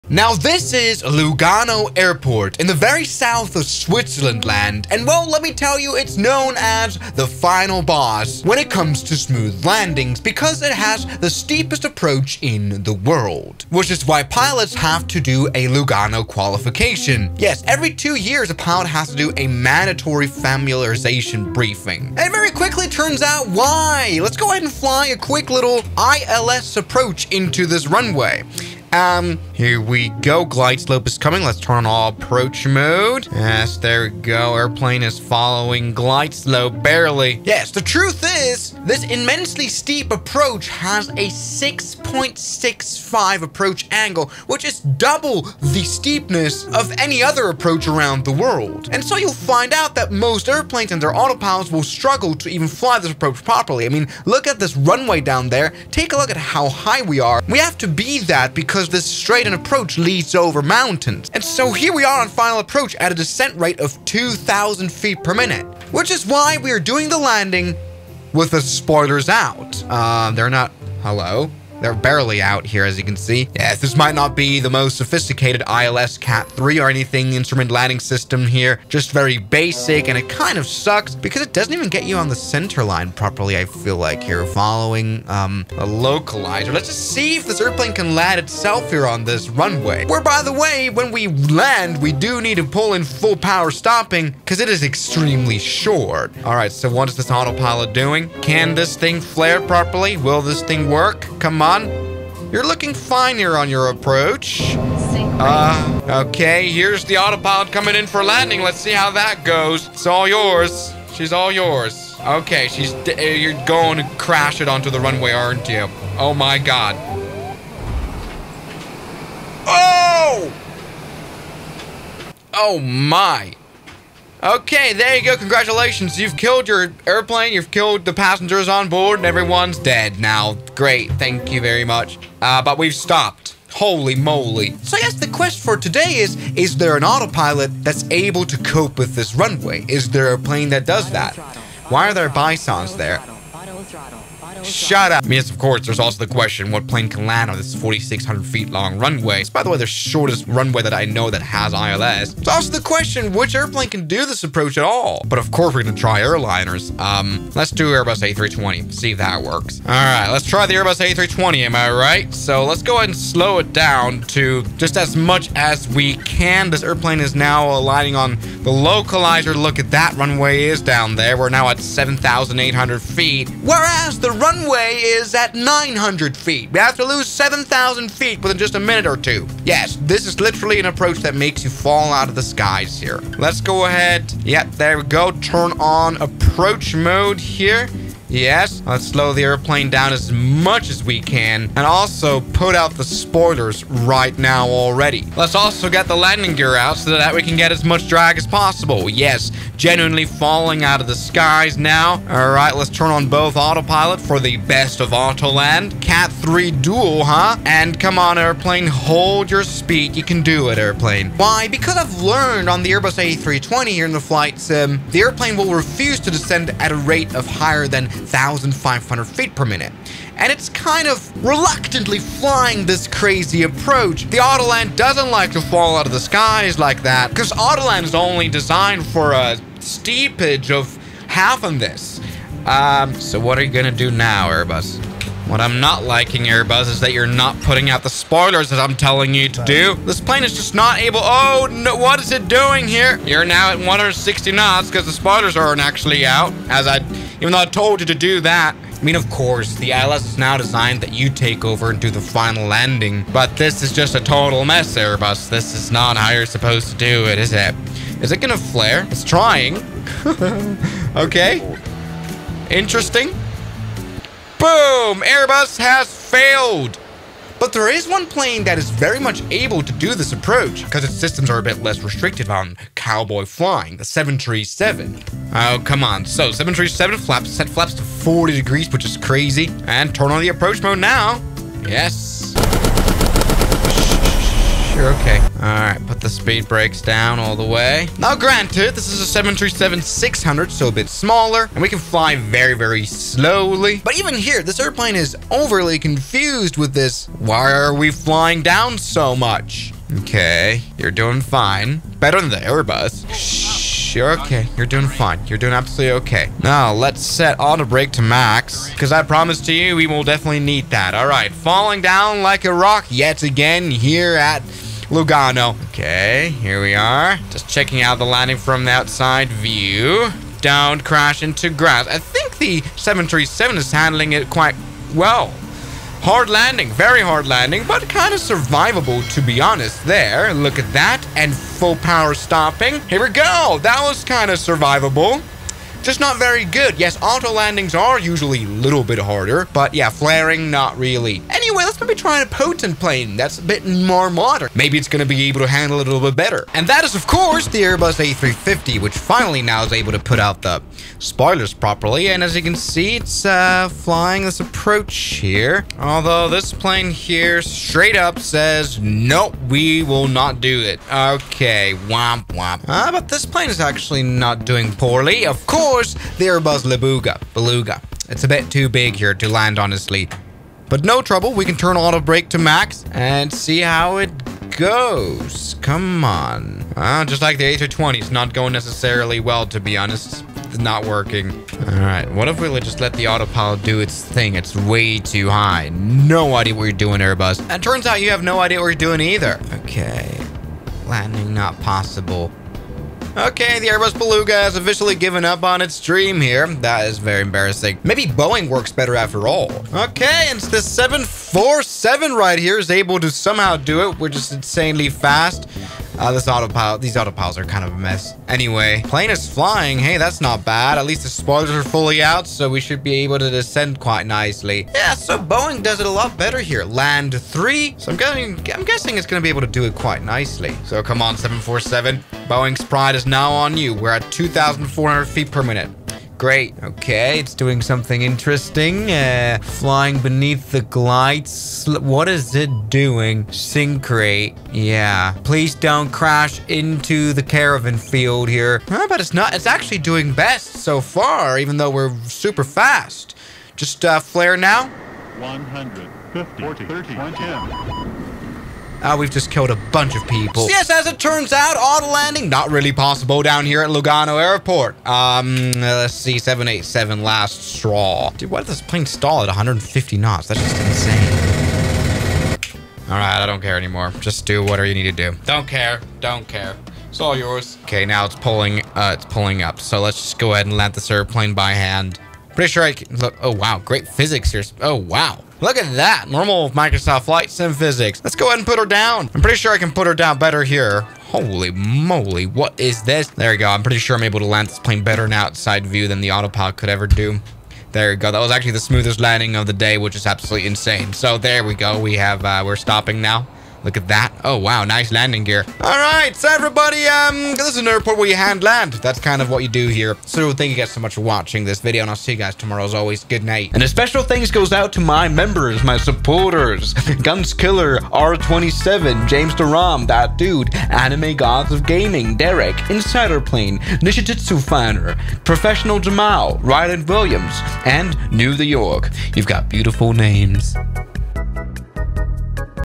Autopilot AUTOLAND Vs The World's STEEPEST APPROACH
Witness the showdown between Autopilot AUTOLAND and the world's steepest approach in Lugano! Watch as the technology takes on the challenge with precision and skill.

Master Aviation Communication with SayIntentions.AI, my new partner!
Train real ATC radio calls, get feedback, and build confidence fast.

©Swiss001 2024